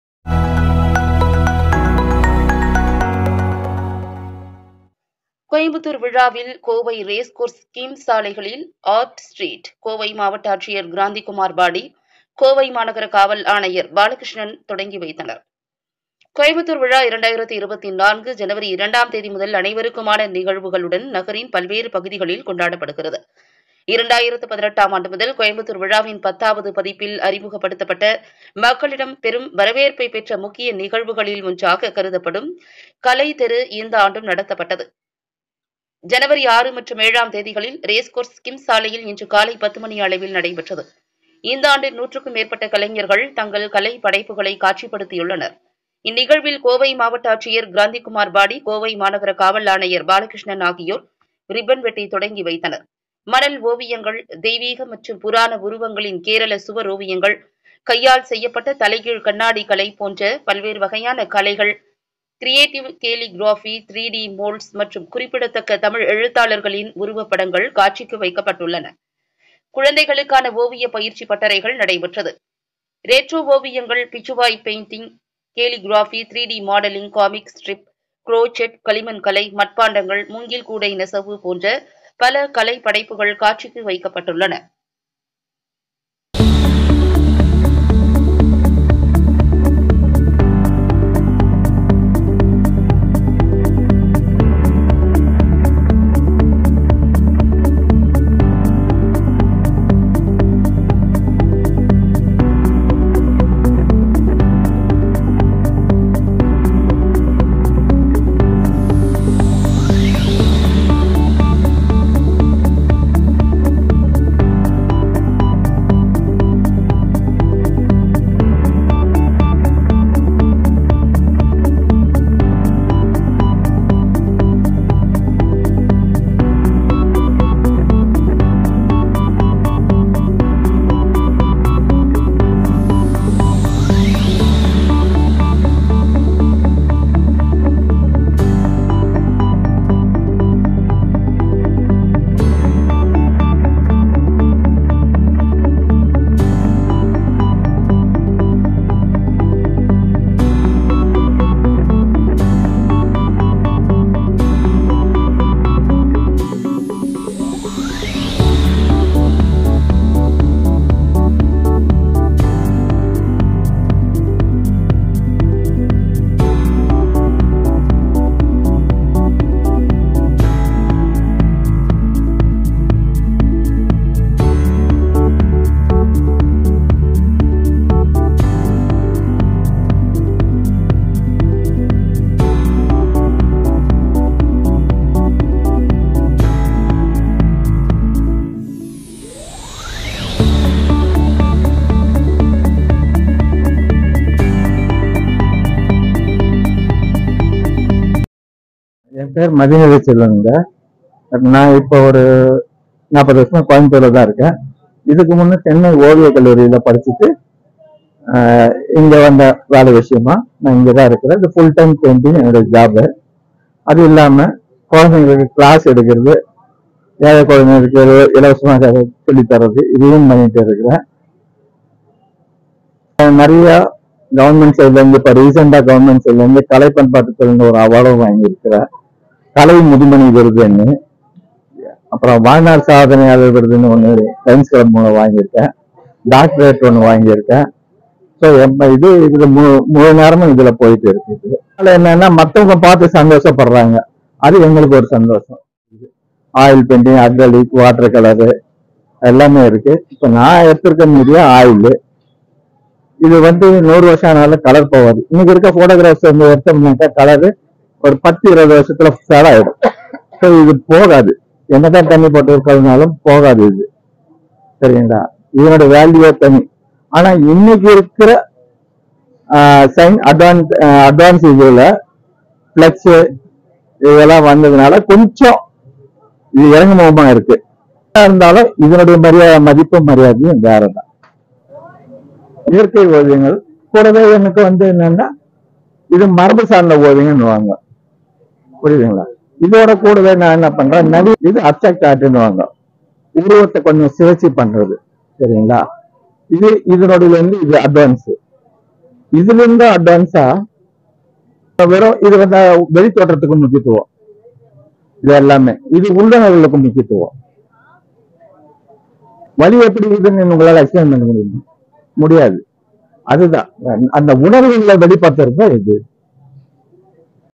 கோயம்புத்தூர் விழாவின் கோவை ரேஸ்கோர்ஸ் ஸ்கீம் சாலைகளில் ஆர்ட் ஸ்ட்ரீட் (ART Street)
கோவை மாவட்ட ஆட்சியர் கிராந்தி குமார் பாடி, கோவை மாநகர காவல் ஆணையர் பாலகிருஷ்ணன் தொடங்கி வைத்தனர்.

கோயம்புத்தூர் விழா 2024 ஜனவரி 2 ஆம் தேதி முதல் அனைவருக்குமான   நிகழ்வுகளுடன் நகரின் பல்வேறு பகுதிகளில் கொண்டாடப்படுகிறது. 2018 ஆம் ஆண்டு முதல் கோயம்புத்தூர் விழாவின் 10 வது பதிப்பில் அறிமுகப்படுத்தப்பட்ட மக்களிடம் பெரும் வரவேற்பை பெற்ற முக்கிய  நிகழ்வுகளில் ஒன்றாக கருதப்படும் கலை தெரு (Art  street ) , இந்த ஆண்டும் நடத்தப்பட்டது.

ஜனவரி 6 மற்றும் 7ம் தேதிகளில் ரேஸ் கோர்ஸ் ஸ்கிம் சாலையில் இன்று காலை 10 மணி அளவில் நடைபெற்றது. இந்த ஆண்டு,  100க்கும் மேற்பட்ட கலைஞர்கள் தங்கள் கலைப்படைப்புகளை காட்சிப்படுத்தியுள்ளனர். இந்நிகழ்வில்  கோவை மாவட்ட ஆட்சியர் கிராந்தி குமார் பாடி, கோவை மாநகர காவல் ஆணையர் பாலகிருஷ்ணன் ரிப்பன் வெட்டி தொடங்கி வைத்தனர்.

மணல் ஓவியங்கள் ,தெய்வீக மற்றும் புராண உருவங்களின் கேரள சுவரோவியங்கள், கையால் செய்யப்பட்ட தலைகீழ் கண்ணாடி கலை போன்ற பல்வேறு வகையான கலைகள், கிரியேட்டிவ் கேலிகிராபி, 3-டி மோல்ட்ஸ் மற்றும் , குறிப்பிடத்தக்க தமிழ் எழுத்தாளர்களின் உருவப்படங்கள் மற்றும் பல காட்சிக்கு வைக்கப்பட்டுள்ளது. குழந்தைகளுக்கான ஓவிய பயிற்சி பட்டறைகள் நடைபெற்றது.

ரெட்ரோ ஓவியங்கள் , பிச்சுவாய் பெயிண்டிங், கேலிகிராபி, 3டி மாடலிங், காமிக் ஸ்ட்ரிப், குரோச்செட், களிமண் கலை, மட்பாண்டங்கள், மூங்கில் கூடை நெசவு போன்ற பல கலைப் படைப்புகள் என பல காட்சிக்கு வைக்கப்பட்டுள்ளது.